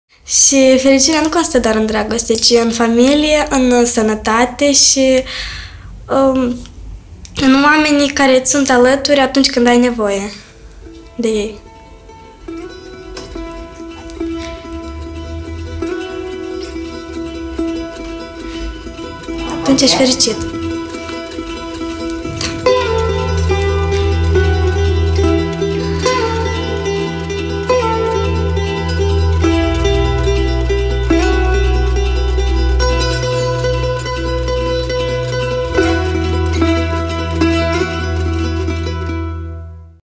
Valeria
”Fericirea nu constă doar în dragoste, ci și în familie, sănătate și în oamenii care-ți sunt alături atunci când ai nevoie.”
«Счастье – это не только любовь, но и семья, здоровье и люди, которые поддерживают, когда тебе это необходимо.»
Valeria a învins boala datorită oamenilor cu inimă mare. Dar mulți alți copii au nevoie de ajutor.